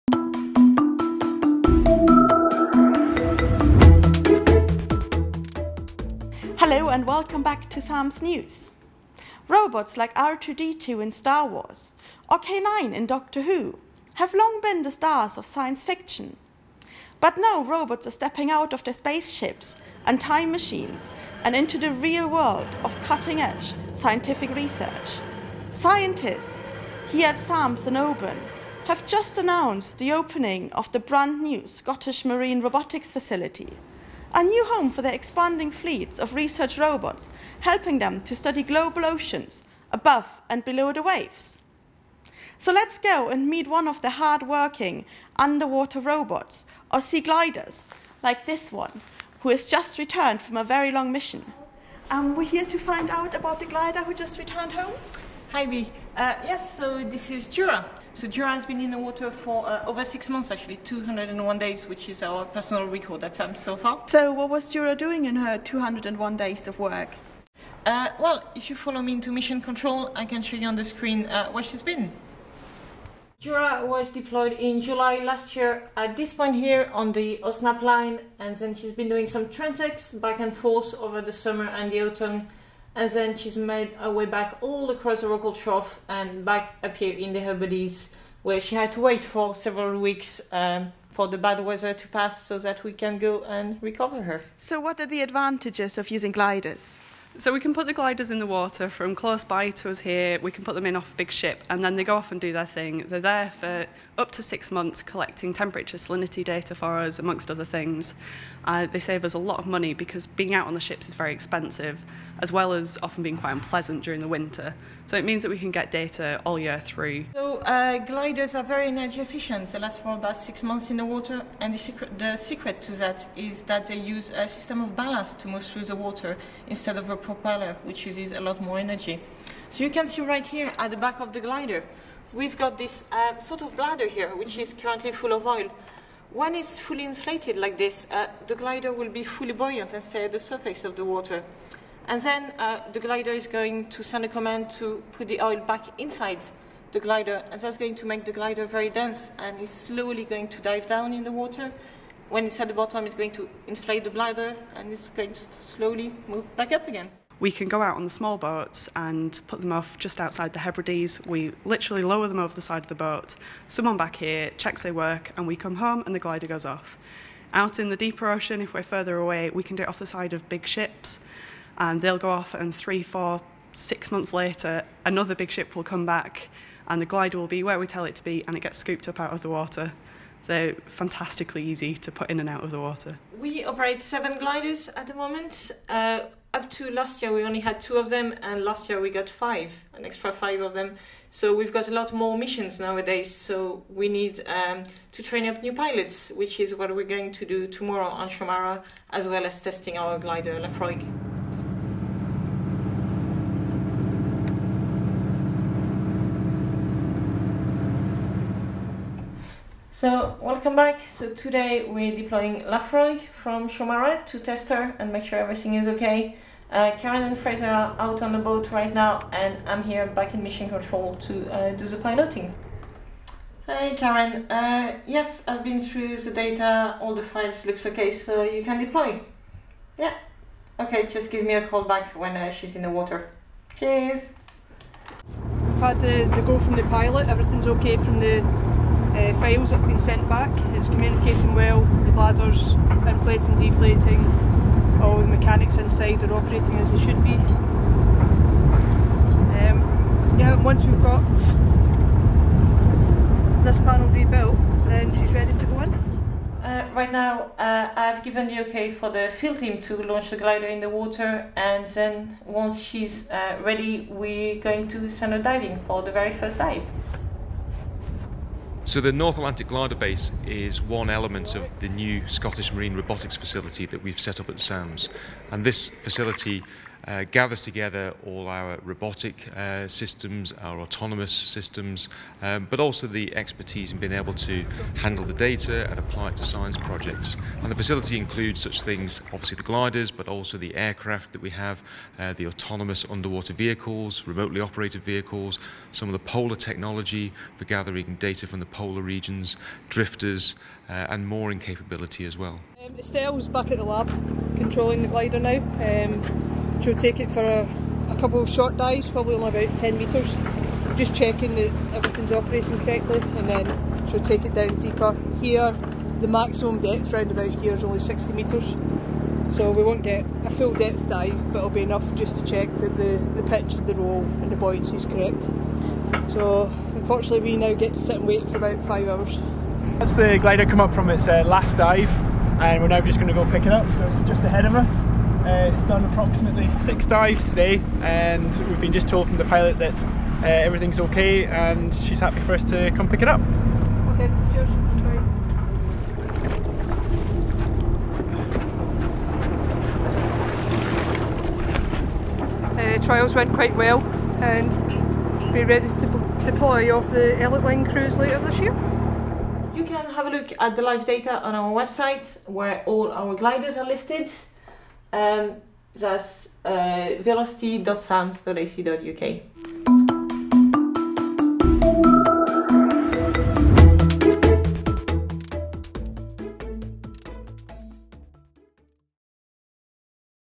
SAMS News - Marine Robots
SAMS-News visits the brand new Scottish Marine Robotics Facility to meet a hard working marine robot called Jura (a Seaglider) who has just returned home to Oban after 201 days collecting data  in the Atlantic , and to find out what SAMS' expanding team of Seaglider pilots and the other six Seagliders are up to.